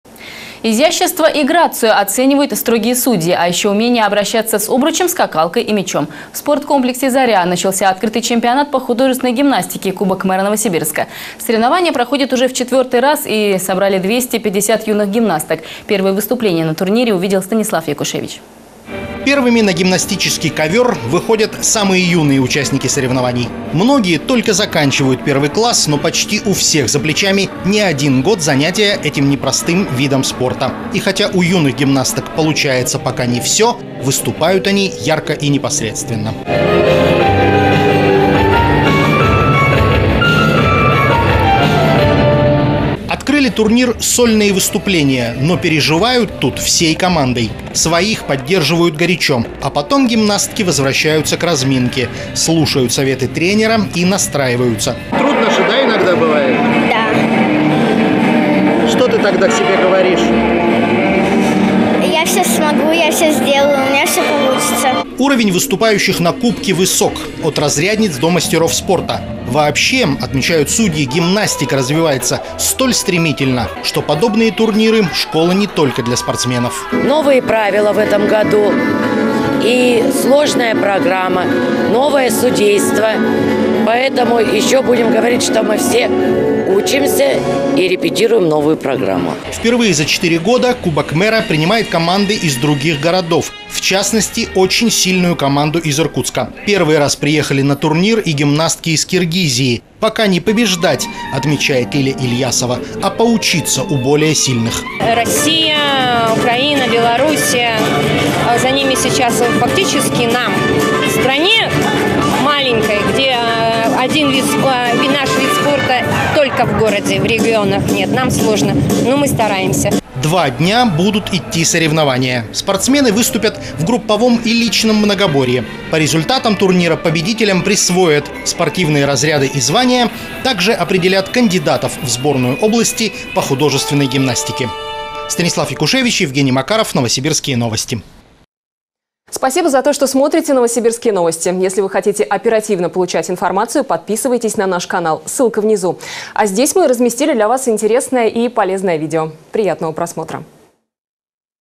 Чемпионат по художественной гимнастике проходит в Новосибирске
Изящество и грацию оценивают строгие судьи, а ещё умение обращаться с обручем, скакалкой и мячом. В спорткомплексе «Заря» начался открытый Чемпионат по художественной гимнастике «Кубок мэра Новосибирска». Соревнования проходят уже в четвертый раз. В индивидуальных и групповых упражнениях поучаствуют двести пятьдесят юных гимнасток из детско-юношеских спортивных школ и центров олимпийской подготовки.